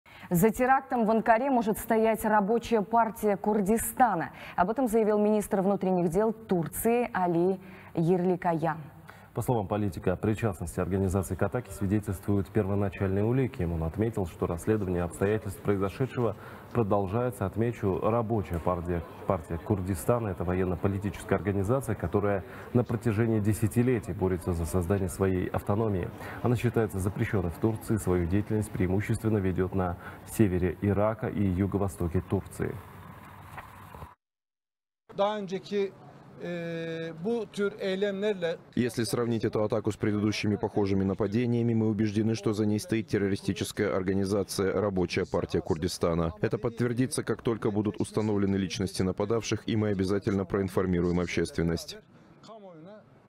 МВД Турции выяснили, кто мог стоять за терактом в Анкаре
За терактом в Анкаре может стоять «Рабочая партия Курдистана». Об этом заявил министр внутренних дел Турции Али Ерликая, передает «24KZ».